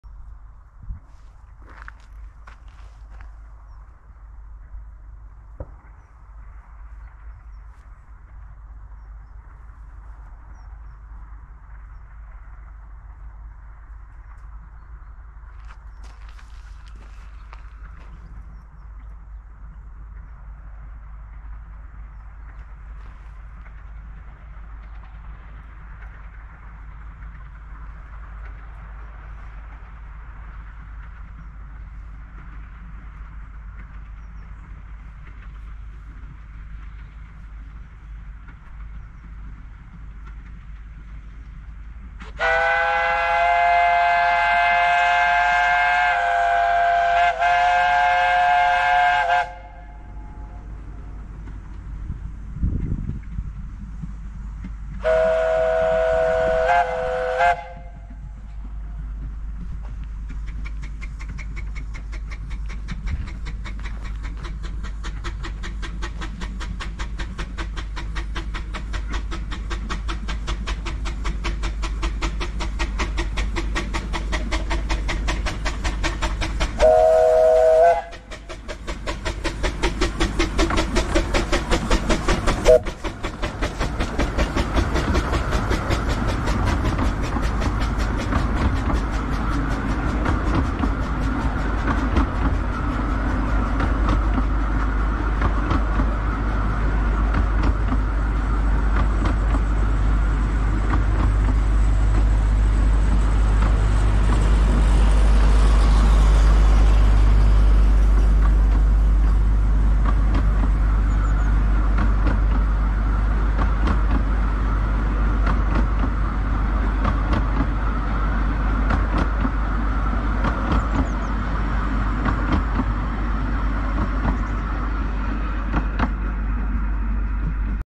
BVR6 passes Spratts Green on a Santa Special with 4 toots
@trainswithlewis8  Driving BVR6